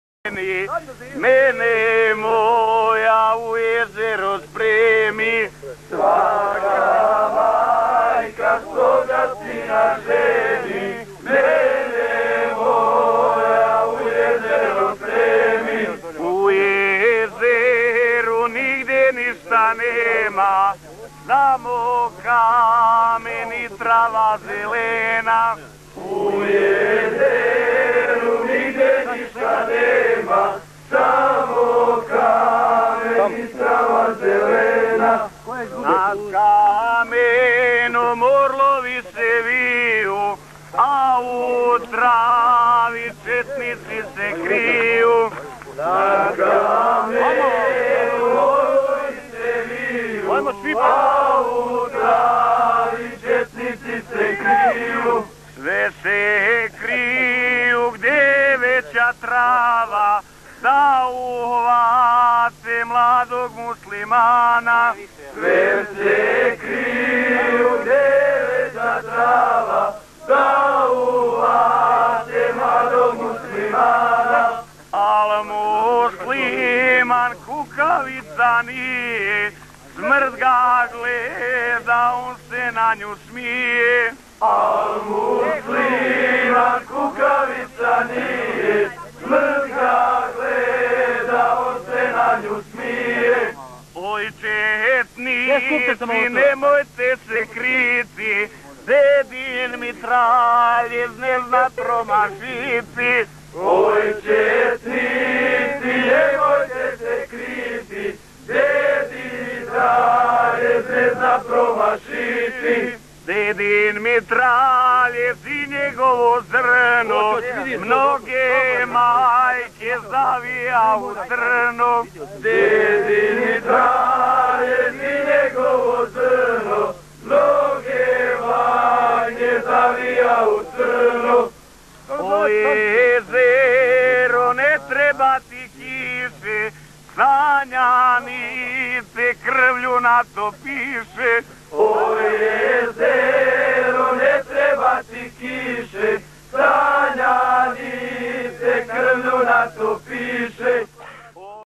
"Heroji Grmuško-Srbljanskog platoa". Borci Ključko-Sanske jedinice
Selo Jezero 1992.godina.

Na snimku rahmetli Ferid Arapović-Fera.

U osvit agresije na BiH, pridružuje se Patriotskoj ligi, sudionik je Galaje i legendarne pobjede sanskih patriota nad višestruko nadmoćnijim neprijateljem. Skupa s glavninom snaga sansko ključke čete, izvukao se na slobodnu bihaćku teritoriju i nekoliko godina poslije, kao pripadnik 510. brigade Petog korpusa, pobjednički umarširao u oslobođeni Sanski Most.

Neka je vječni rahmet i laka Bosanska zemlja herojima. @bestvideos8877